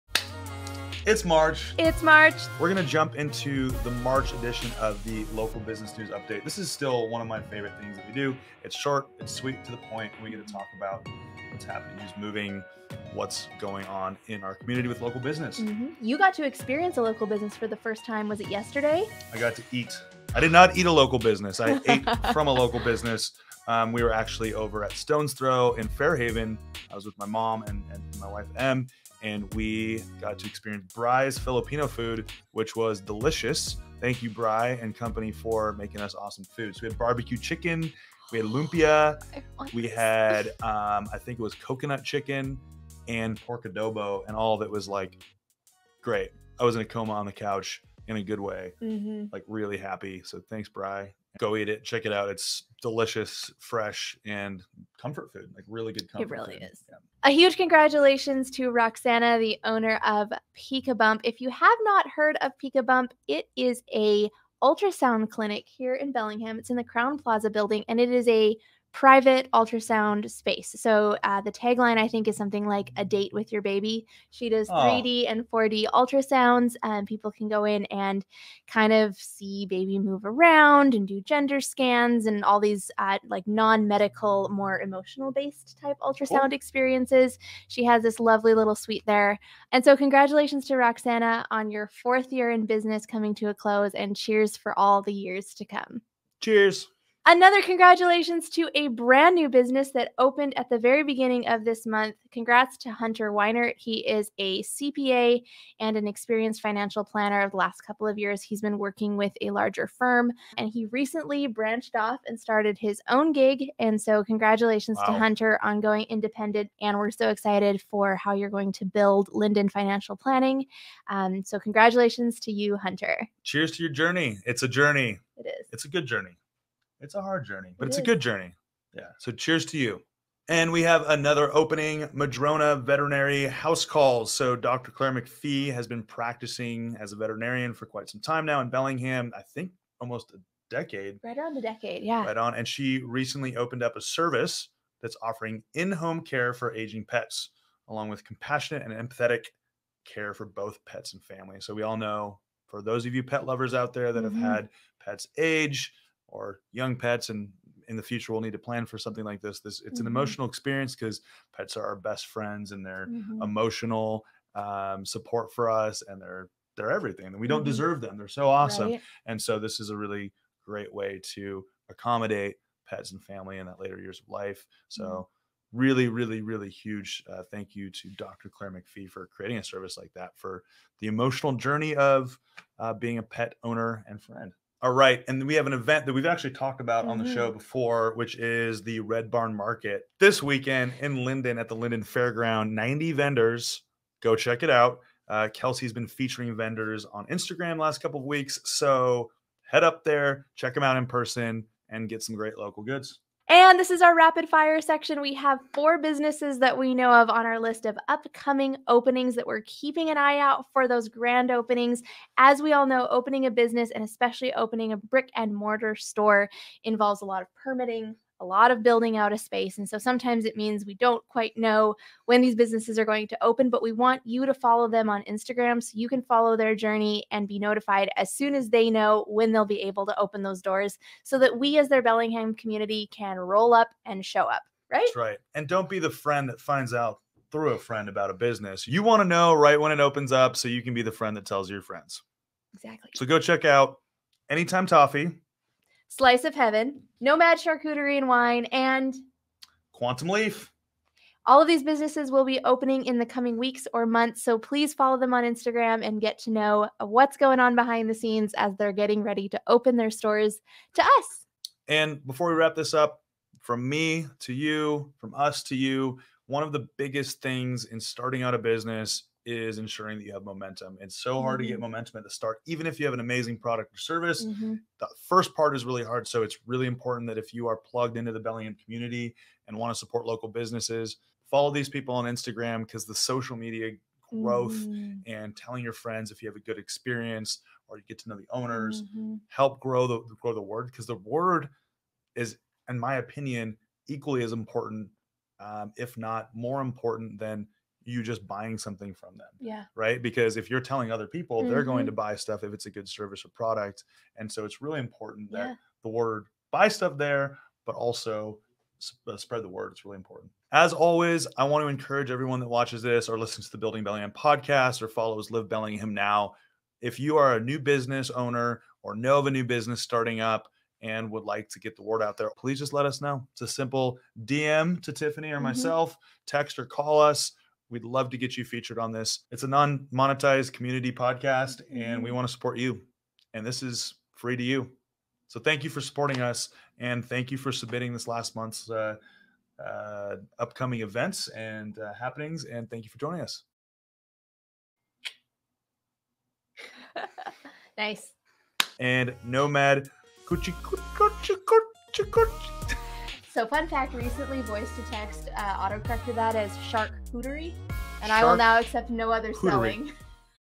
March | Local Business News Update
We're BACK with the March Business News Update! Leo & Tiffany bring you local grand openings, reopenings and relocations each and every month.